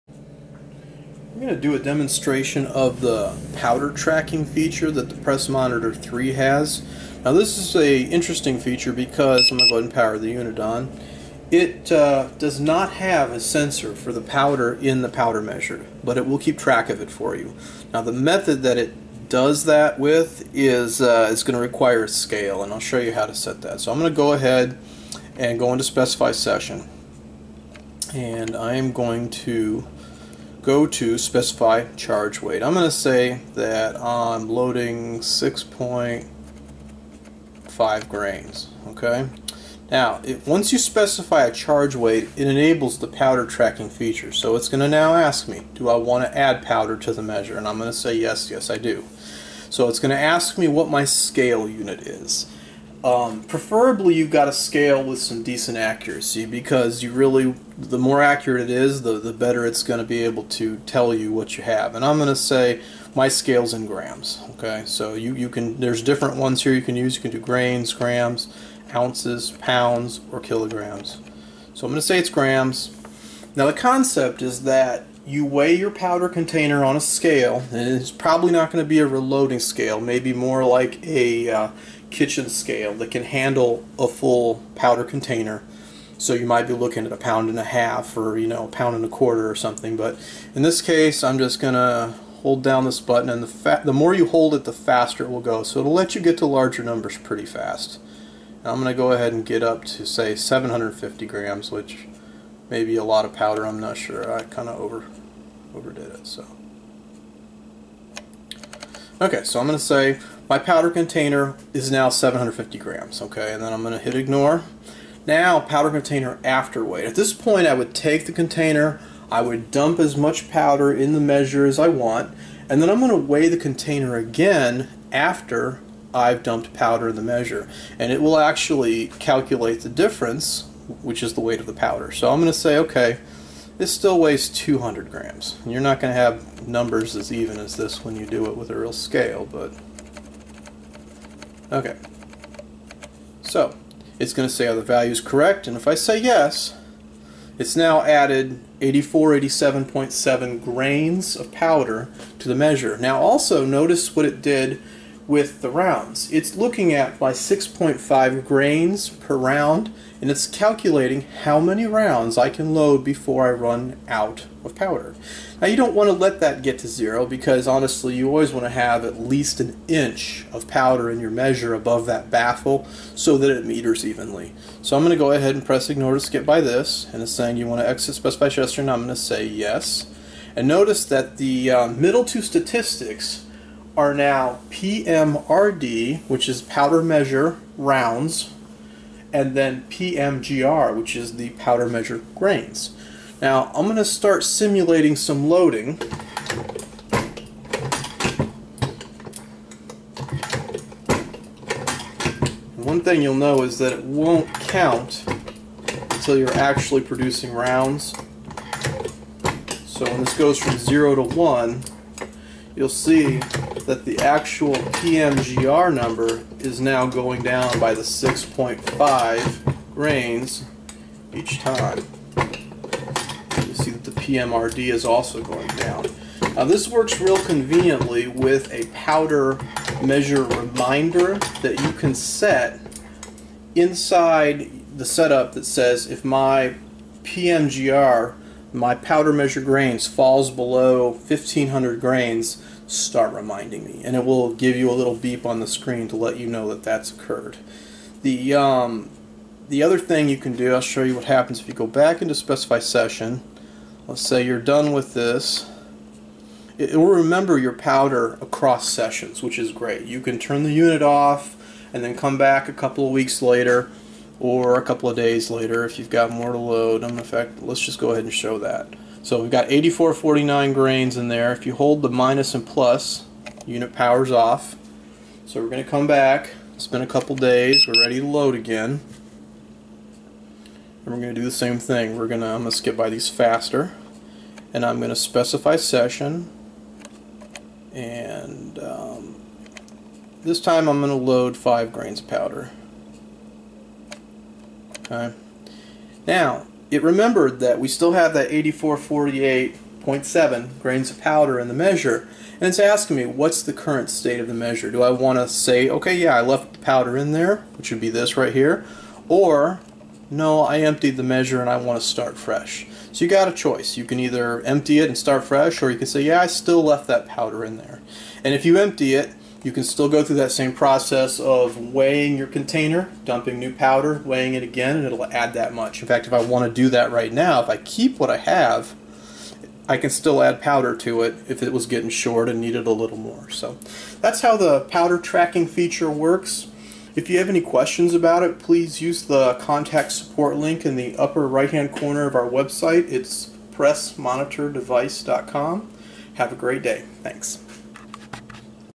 Press Monitor III Powder Tracking
This gives an example of how the powder tracking feature in the Press Monitor III works.